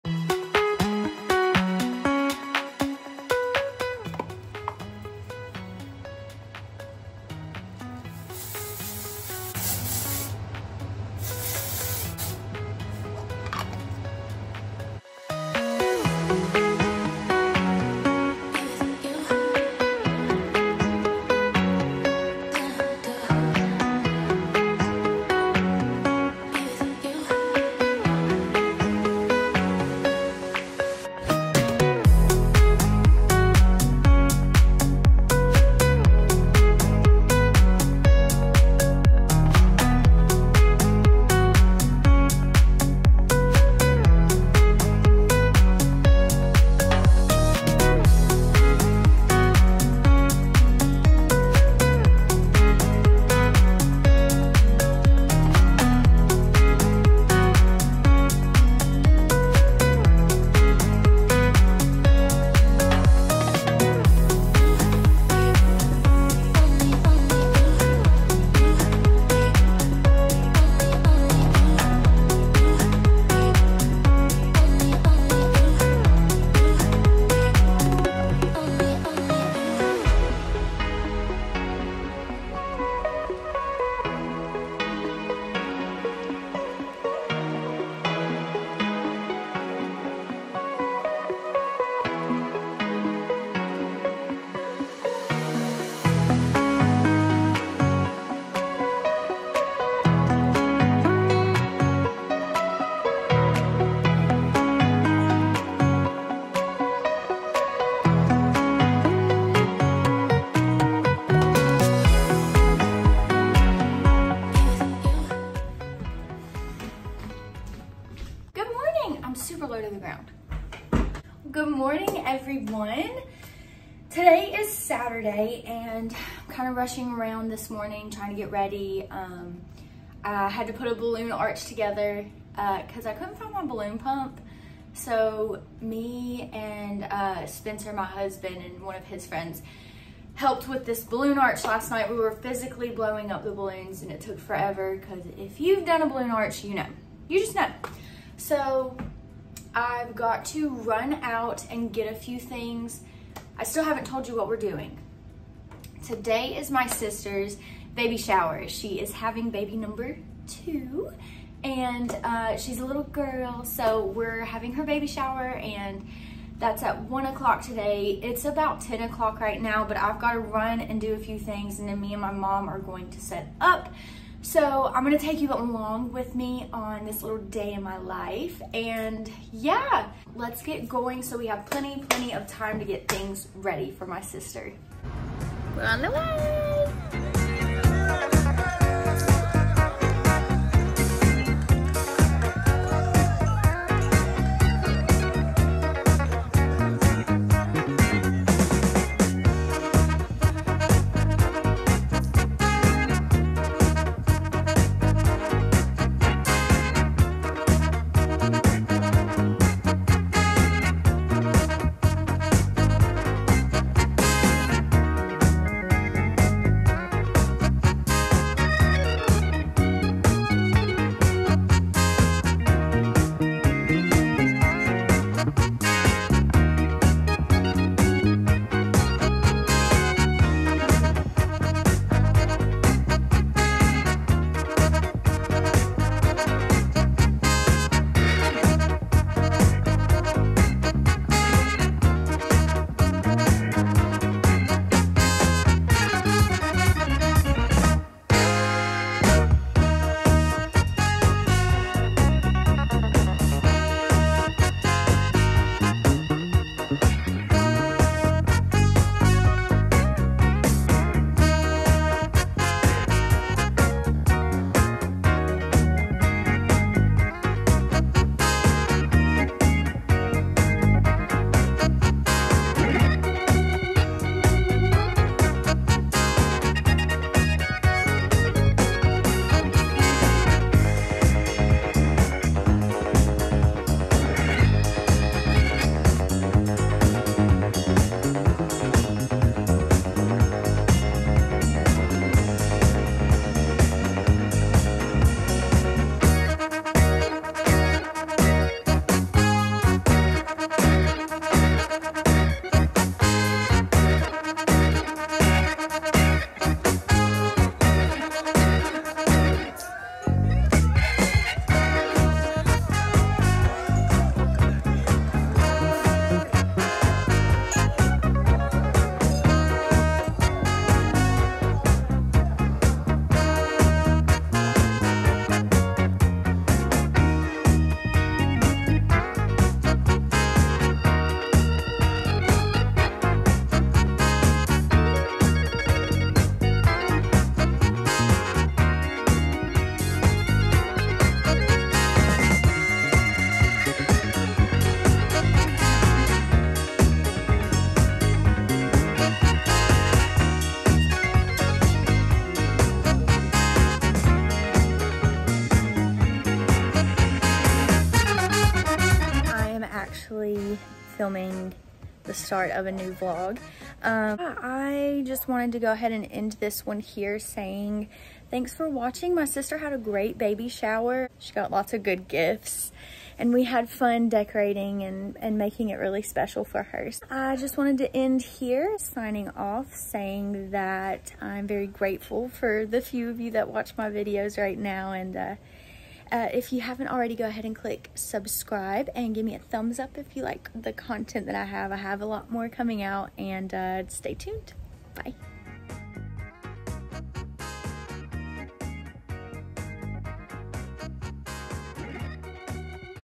MY SISTERS BABY SHOWER || get ready with me & decorating a baby shower
My sister is pregnant with baby #2 and we will have two little girlies running around by August !! her baby shower theme was Under the Sea

my mom, my dad, & me had a fun time decorating together 🩷

WE SHOULD BE FRIENDS !! 🤍